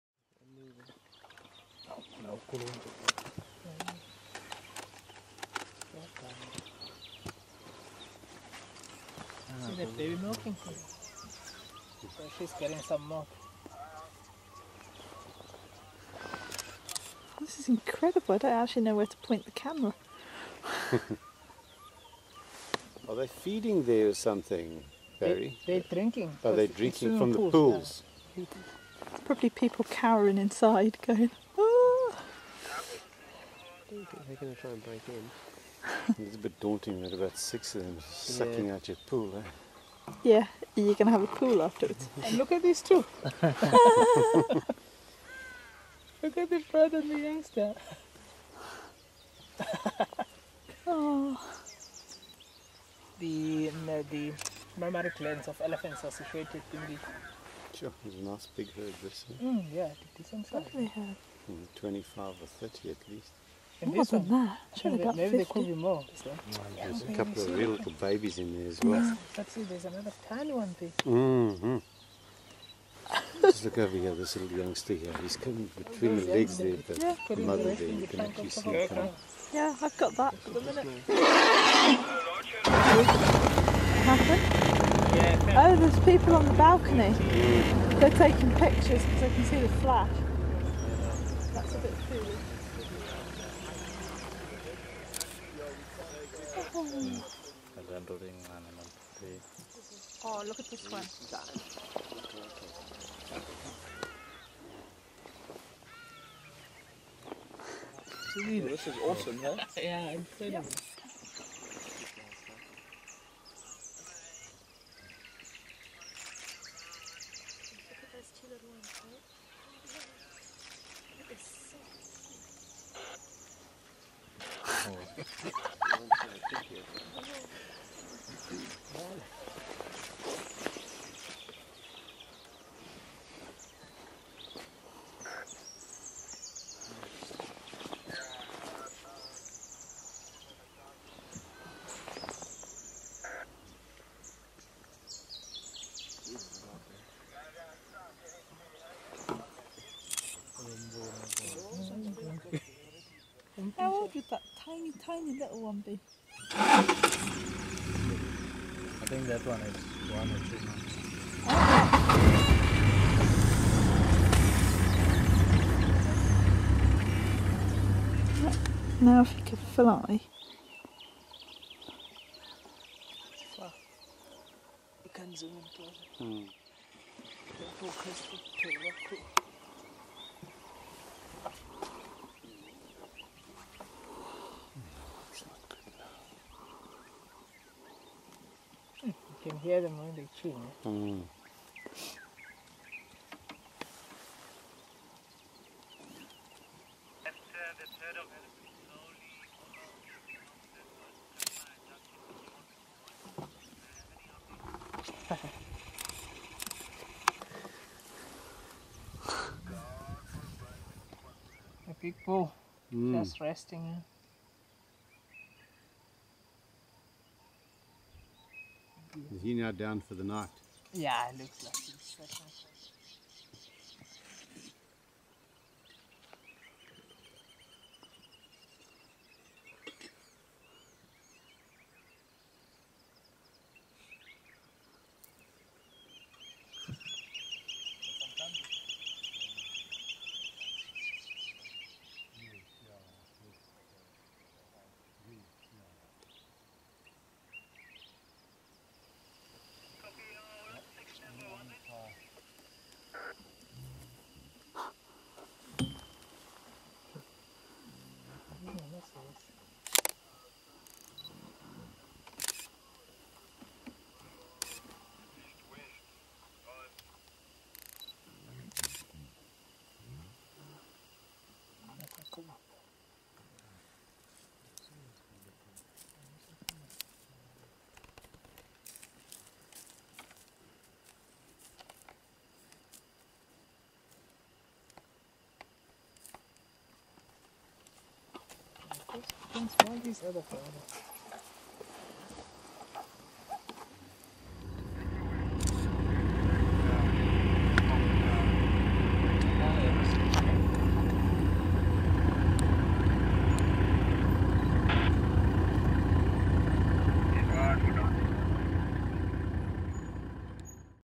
Safari 2011: Afternoon elephants and late-night lion
A herd of elephants find a novel place to take a drink, a white rhino finds a spot to rest for the evening and a lion... er... just kind of rolls around in the dark for a while and then wanders off. An ace evening drive from Phinda Forest Lodge in September 2011.